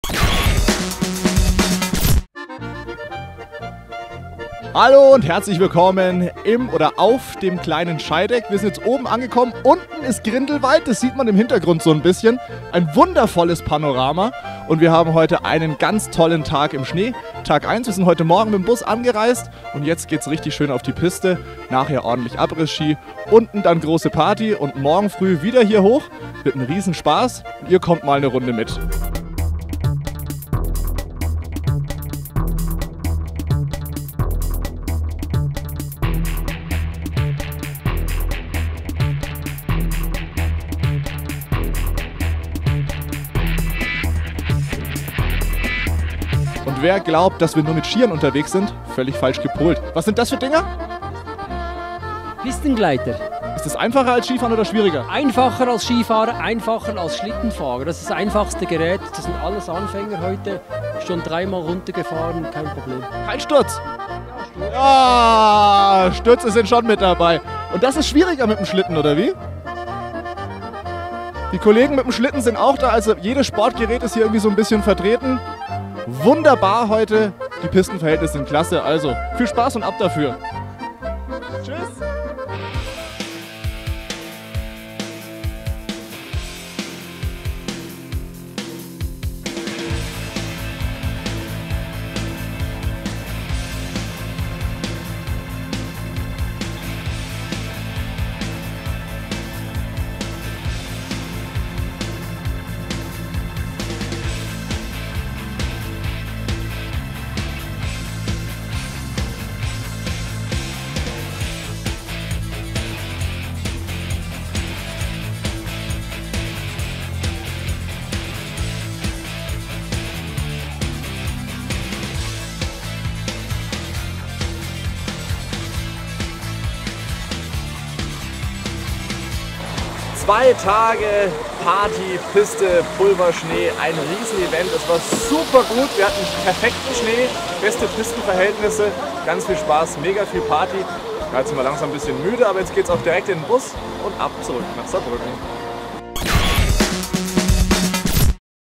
RADIO SALÜ Ski-Opening 2012
Am 15. und 16. Dezember 2012 fuhren wieder viele RADIO SALÜ Hörer mit ins Skigebiet Grindelwald, um dort das RADIO SALÜ Ski-Opening zu feiern.